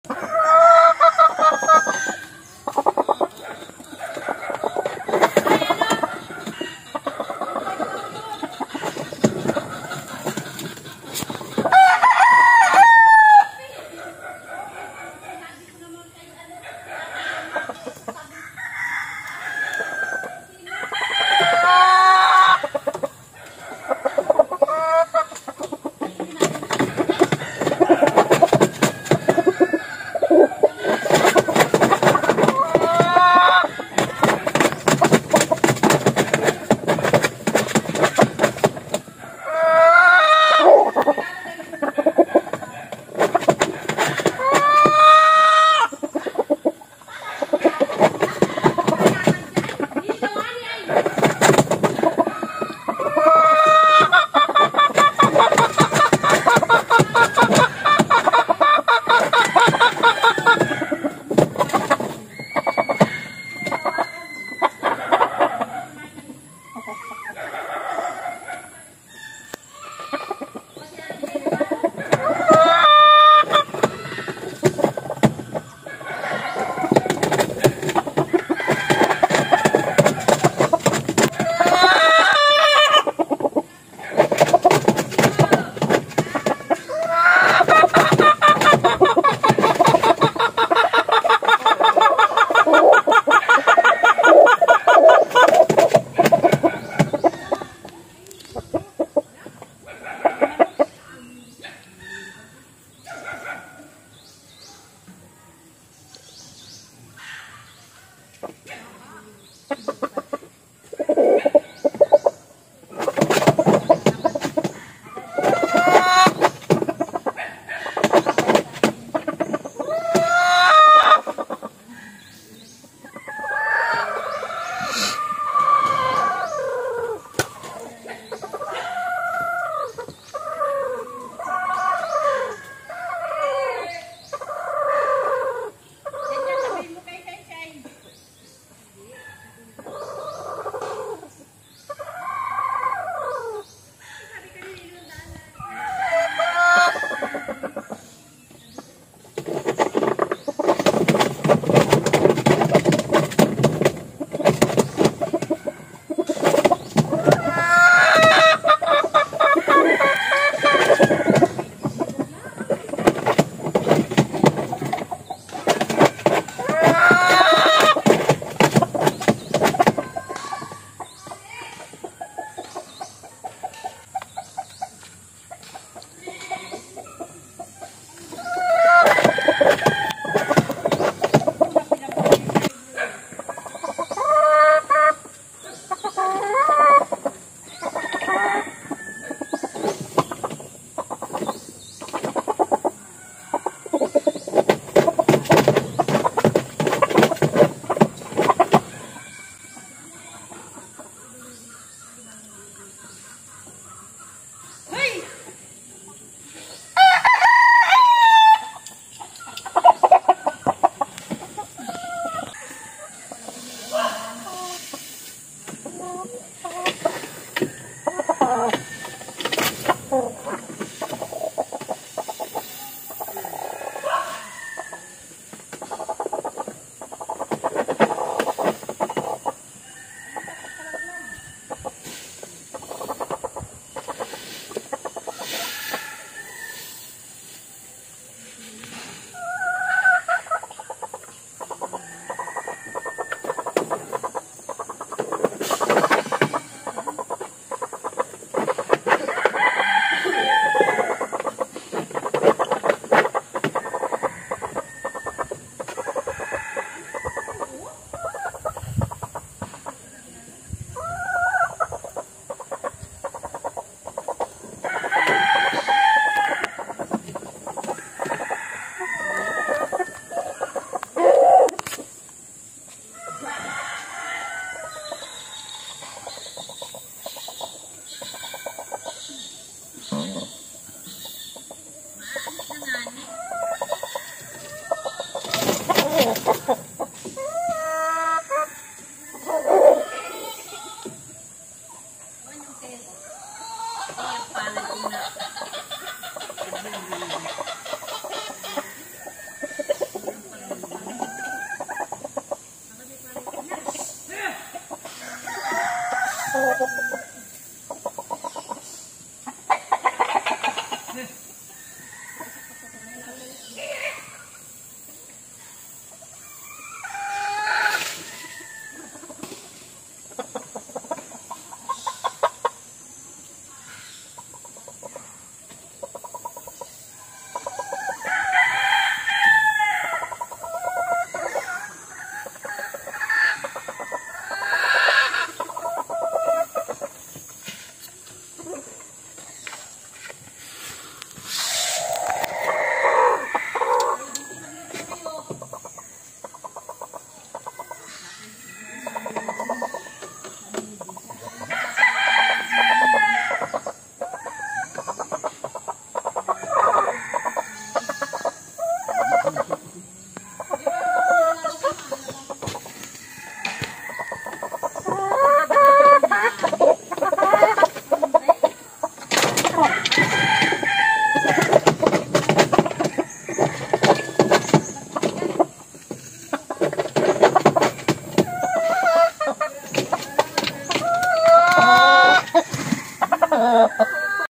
Boston Roundhead ganito pala mag itlog.
Ganito pala mag itlog ang aking Boston Roundhead.